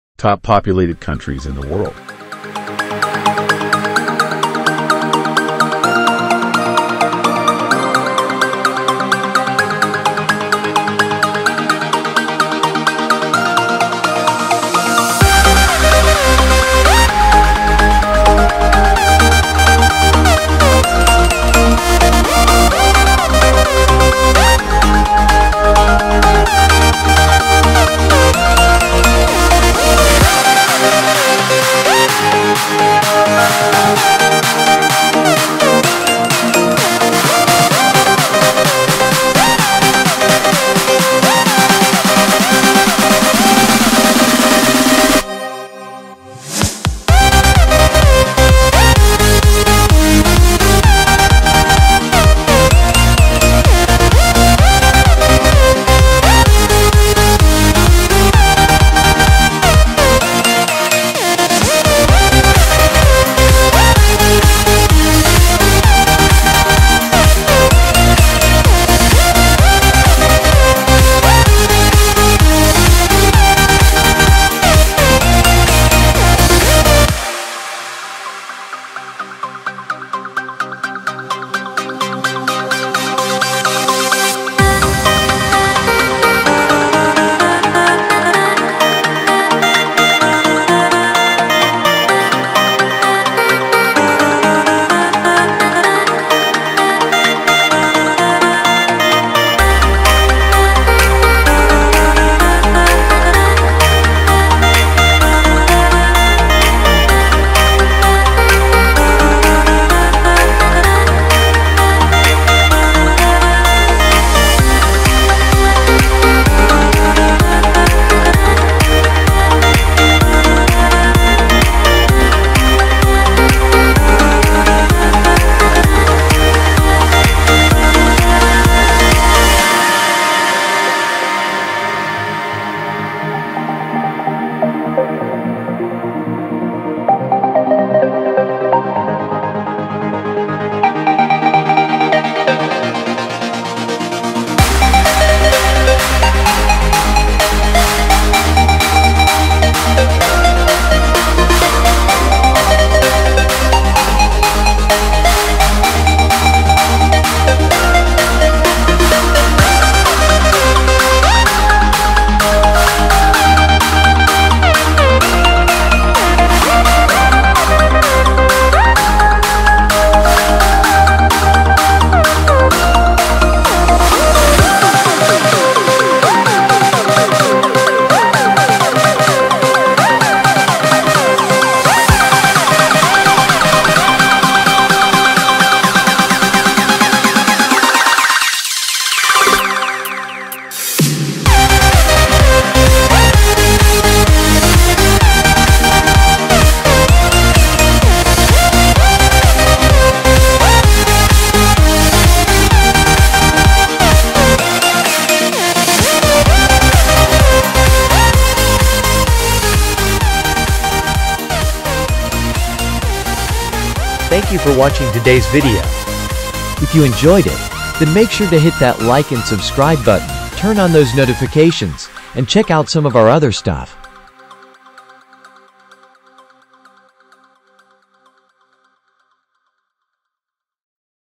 Top Populated Countries 3D
Explore the world like never before in our latest YouTube video, 'Top Populated Countries 3D'! Join us on a visually stunning journey through the most populous nations on Earth, brought to life in immersive 3D graphics. Discover fascinating facts and figures about these diverse countries as we showcase their populations in a whole new dimension. Don't miss out on this captivating and educational experience - hit play now and embark on a virtual tour of the world's most populous places!